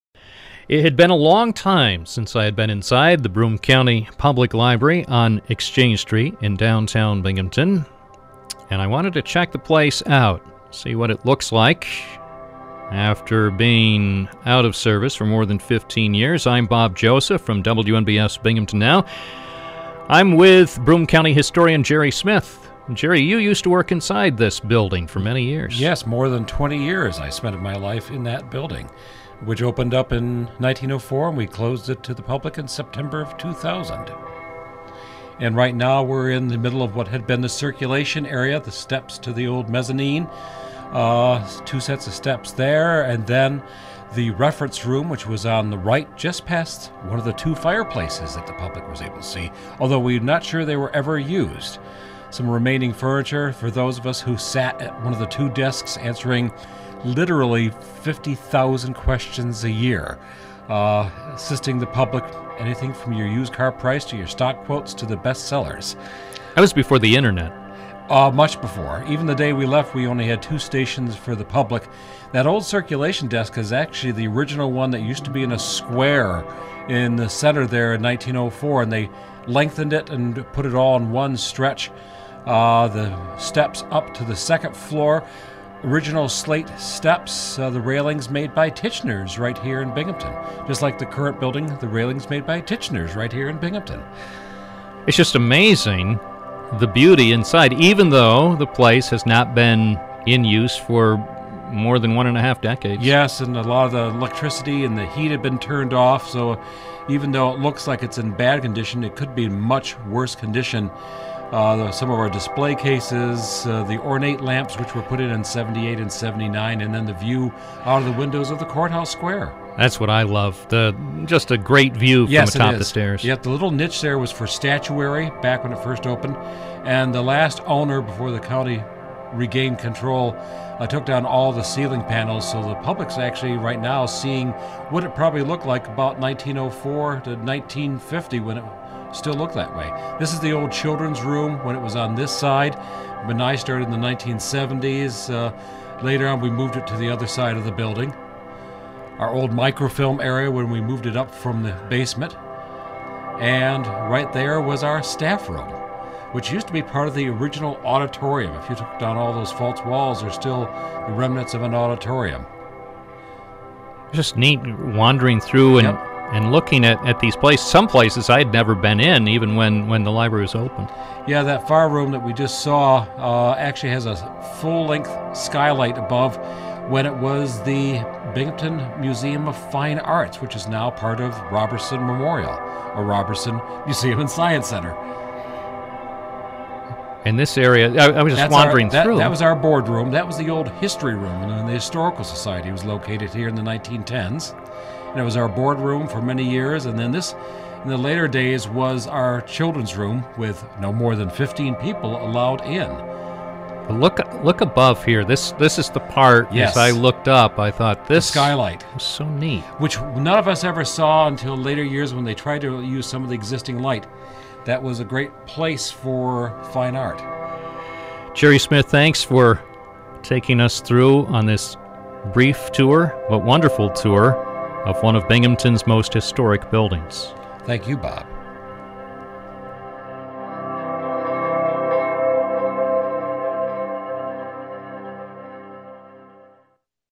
A Tour of Binghamton's Historic Carnegie Library
A rare look inside the Exchange Street building with WNBF "Binghamton Now" host Bob Joseph and Broome County historian Gerald Smith.